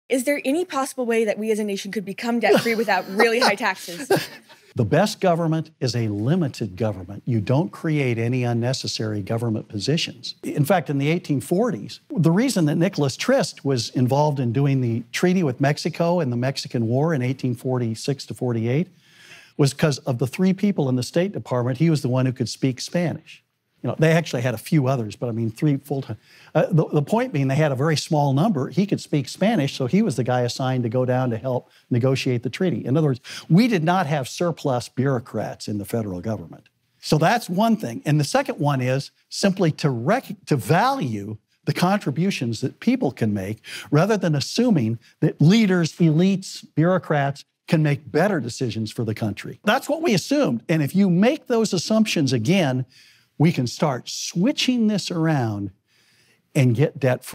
It’s Time To ABOLISH Unnecessary, Corrupt Bureaucratic Positions
A student asked Burt Folsom on how the U.S. can get rid of it's rising debt and what steps must be taken to do so.